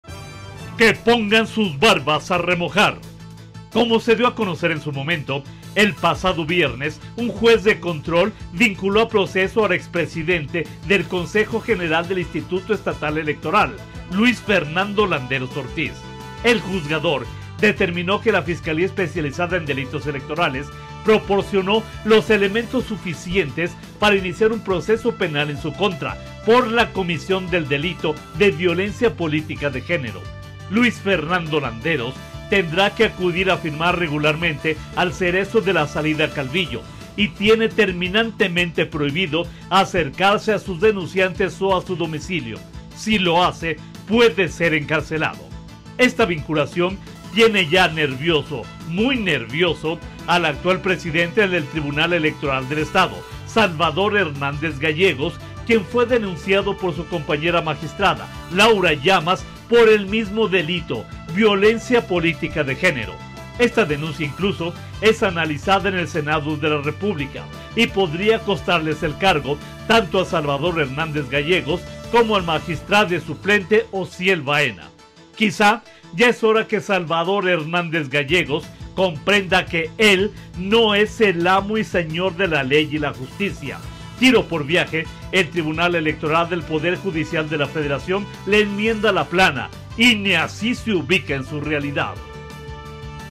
Crónica del Poder/Que pongan sus barbas a remojar
Que pongan sus barbas a remojar

Aguascalientes, Ags. 15 mayo 2023.- Como se dio a conocer en su momento, el pasado viernes un juez de control vinculó a proceso al ex presidente del Consejo General del Instituto Estatal Electoral, Luis Fernando Landeros Ortiz.
El juzgador determinó que la Fiscalía Especializada en Delitos Electorales proporcionó los suficientes elementos para iniciar un proceso penal en su contra por la comisión del delito de Violencia Política de Género.
Luis Fernando Landeros tendrá que acudir a firmar regularmente al Cereso de la salida a Calvillo y tiene terminantemente prohibido acercarse a sus denunciantes o al domicilio. Si lo hace puede ser encarcelado.
Esta vinculación ya tiene nervioso, muy nervioso al actual presidente del Tribunal Electoral del Estado, Salvador Hernández Gallegos, quien fue denunciado por su compañera magistrada Laura Llamas por el mismo delito, Violencia Política de Género. Esta denuncia incluso es analizada en el Senado de la República y podría costarles el cargo tanto a Salvador Hernández como al magistrado suplente Ociel Baena.
Quizá ya es hora que Salvador Hernández Gallegos comprenda que él no es el amo y señor de la ley. Tiro por viaje el Tribunal Electoral del Poder Judicial de la Federación le enmienda la plana y ni así se ubica en su realidad.